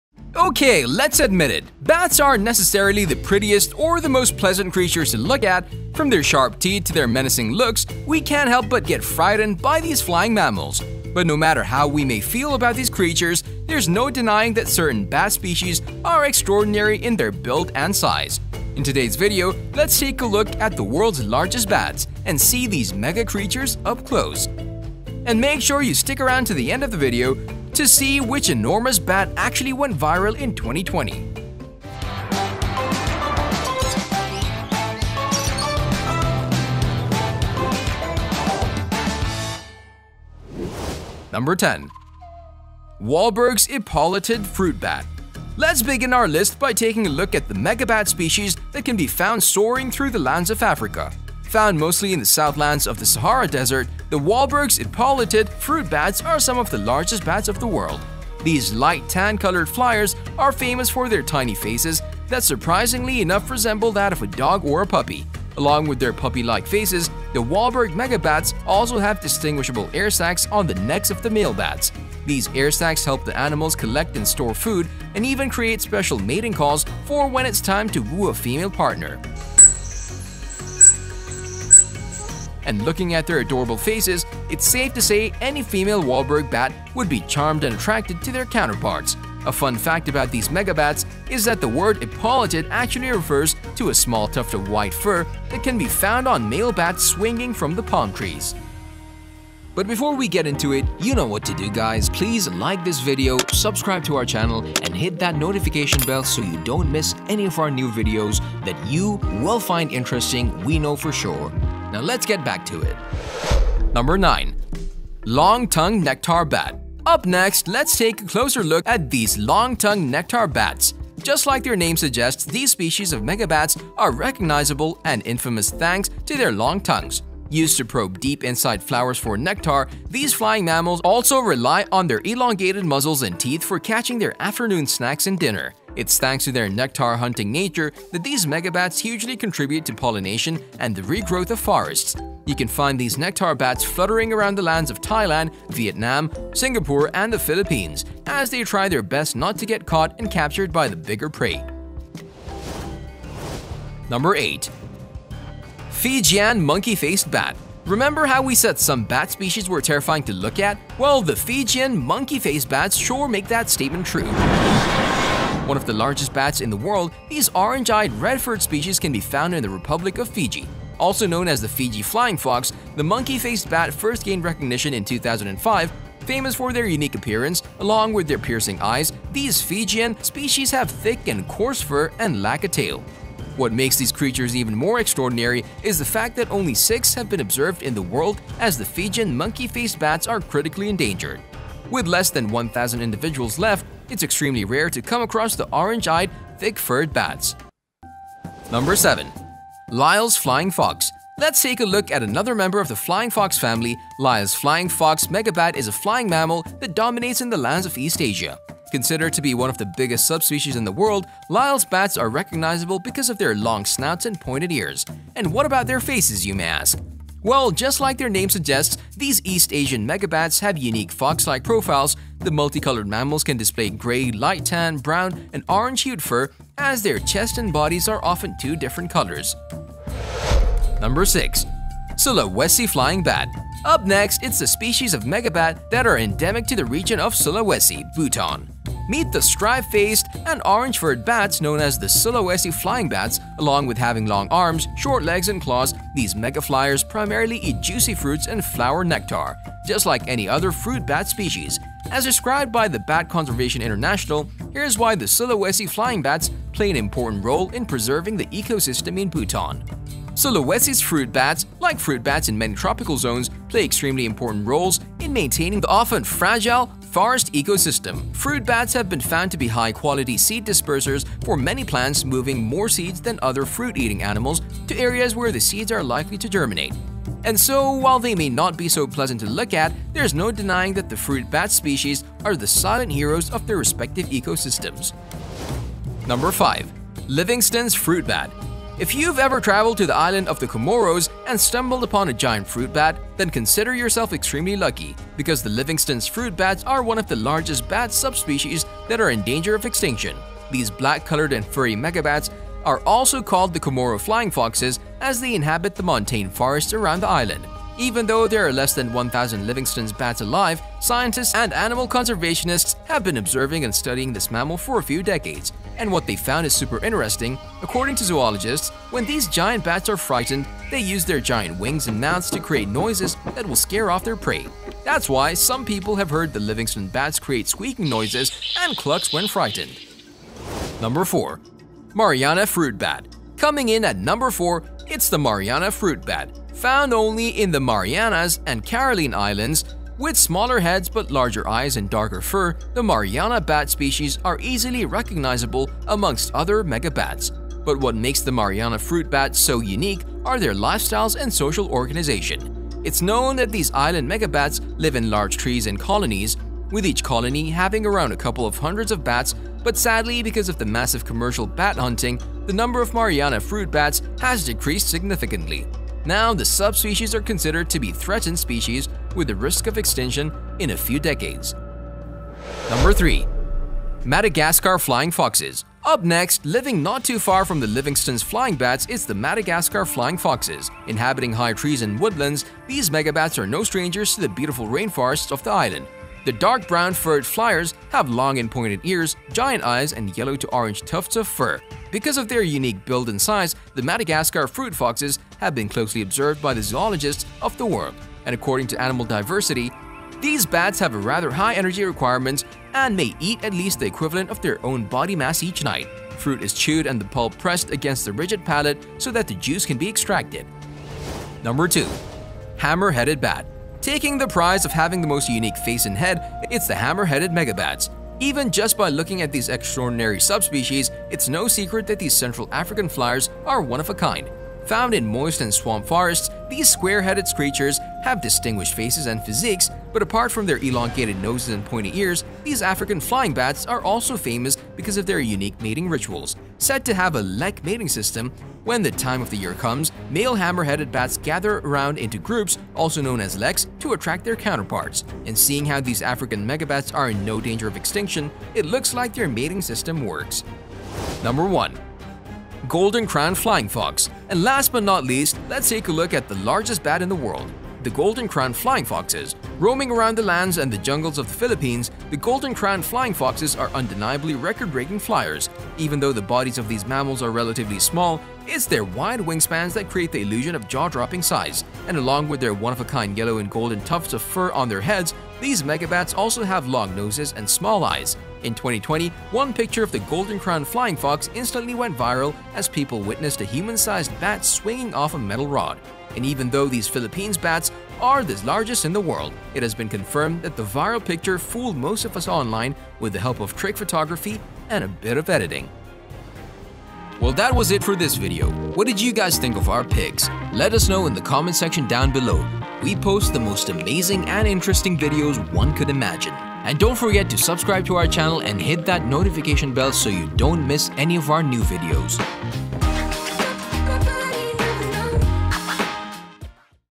Top 10 World's Largest Bats | Golden Crowned Flying Fox | Factsosaurus
Today we will talk about the world's largest bats, actually 10 of the biggest bats the world has. And there are some extreme monsters out there! For exempel there is the giant golden-crowned flying fox! Which is actually human-size! Based in the Philippines? With a wingspan of 1,7 meters, this is not something you would love to encounter now is it?

So let's learn all there is to know about these giant bats!